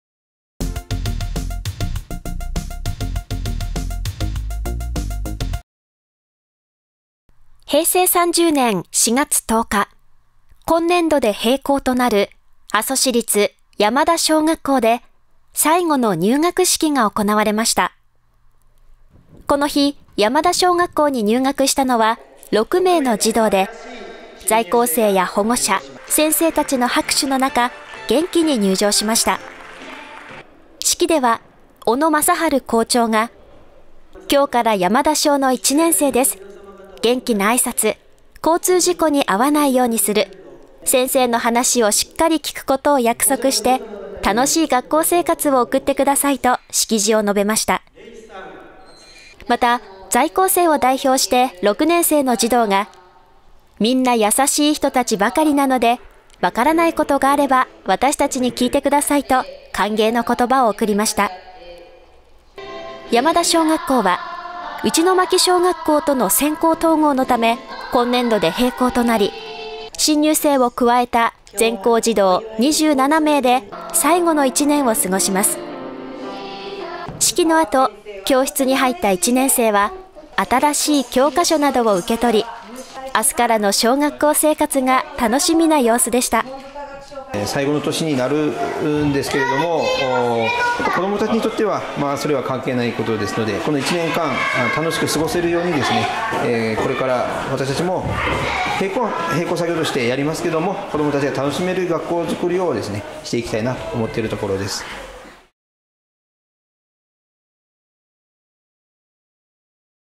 山田小学校で最後の入学式
今年度で閉校となる阿蘇市立山田小学校で最後の入学式が行われました。